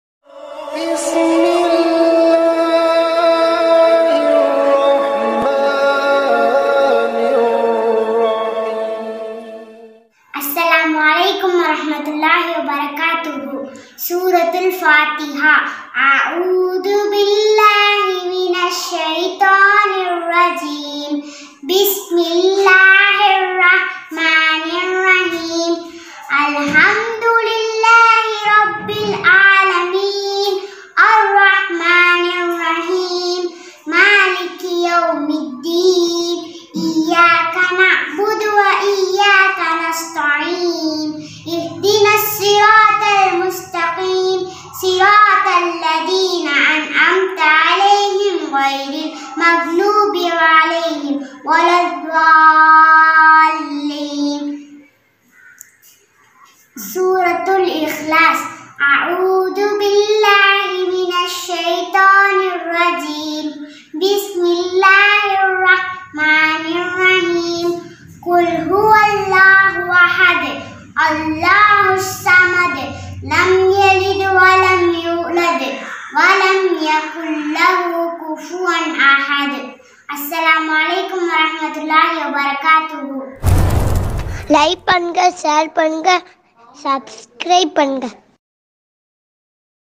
Adirai Islamic Media போட்டியாளர் T.Thasbeeha | A 248 | Adirampattinam |YouTube வழி சூரா ஓதும் போட்டி
ASSALAMU ALAIKUM {WARAH}

Adirai Islamic Media மாபெரும் இரண்டாம் ஆண்டு யூடியூப் வழி சூரா ஓதும் போட்டி

அனைவருக்கும் பகிருங்கள்

தொடர்புக்கு,

Whatsapp Only